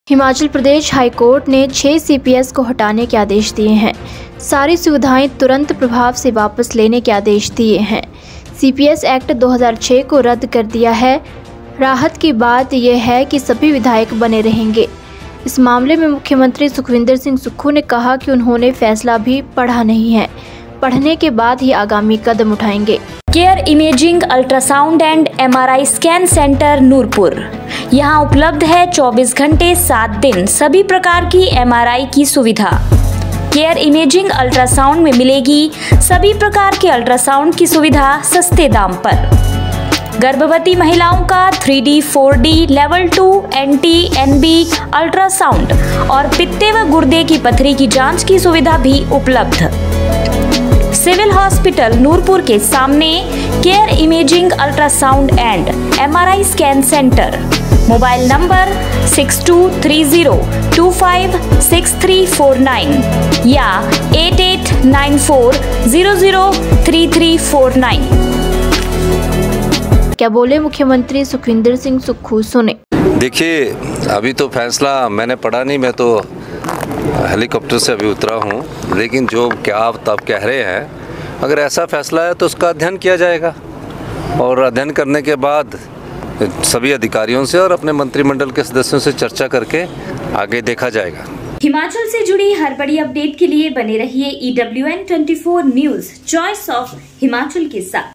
CPS मामले पर क्या बोले सुखविंदर सुक्खू, हाईकोर्ट ने हटाने के दिए हैं आदेश
Himachal Pradesh High Court has ordered the removal of 6 CPS officers. All facilities have been withdrawn with immediate effect. CPS Act 2006 has been repealed. The matter of relief is that all the MLAs will remain.

In this matter, Chief Minister Sukhwinder Singh Sukhu said that he has not read the decision yet. He will take further steps only after reading it.